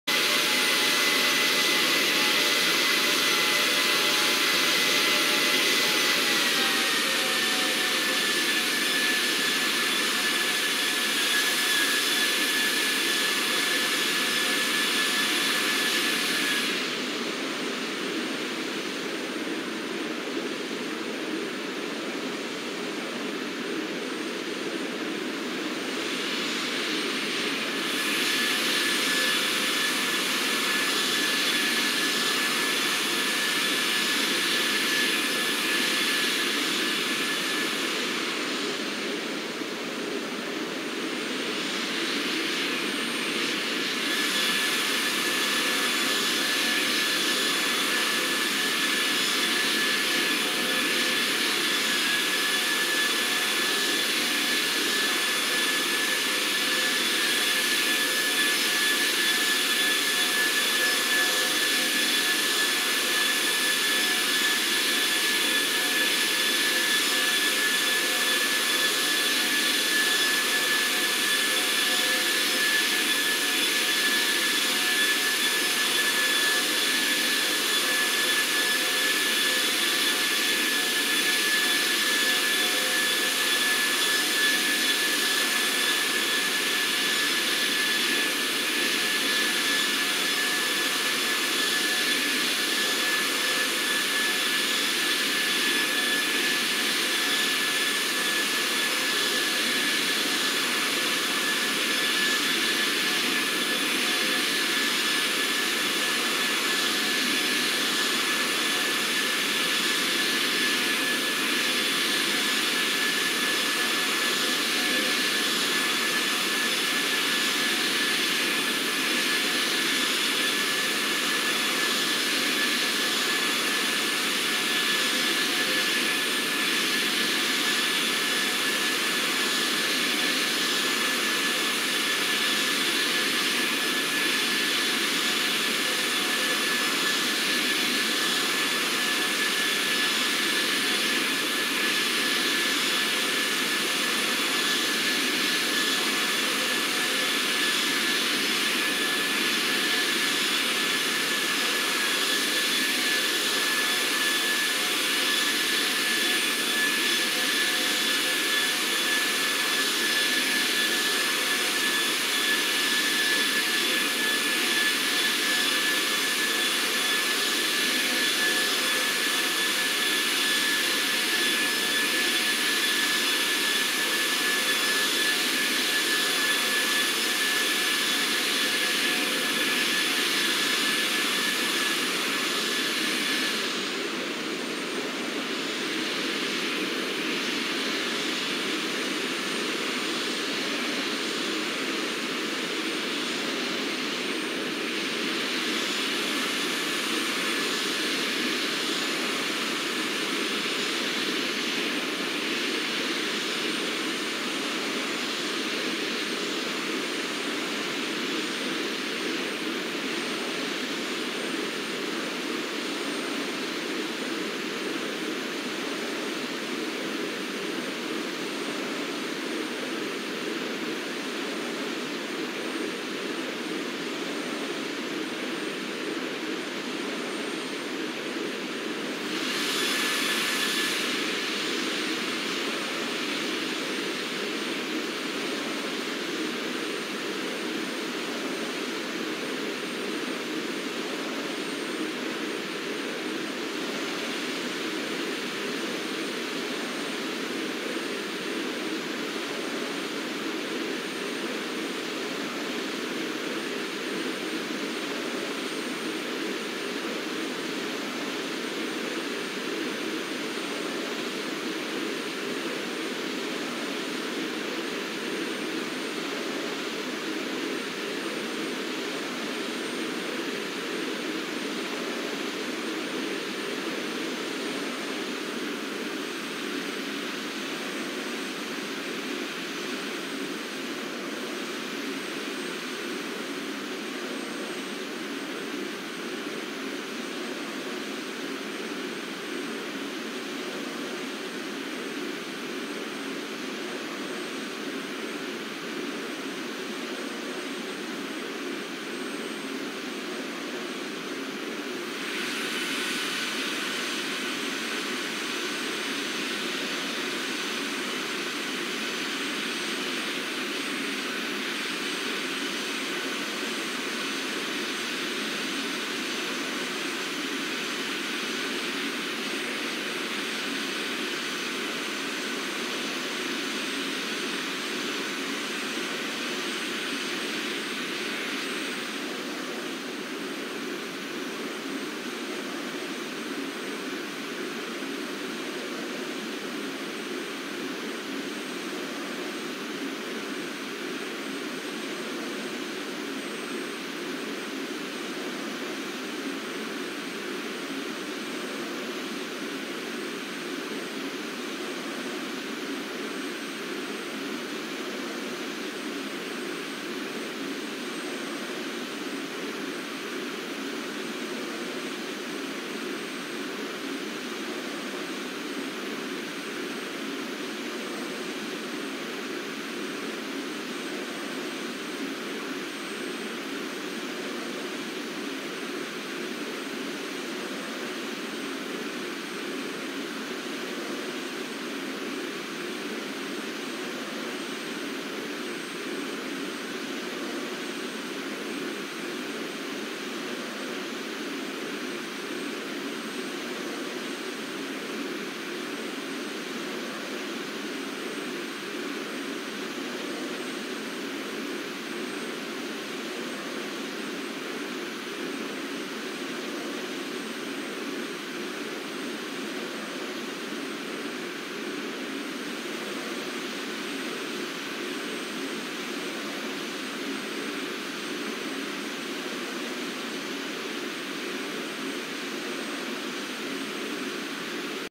Flying Around Vitebsk
Vitebsk is a Belarusian city. my friend lives here.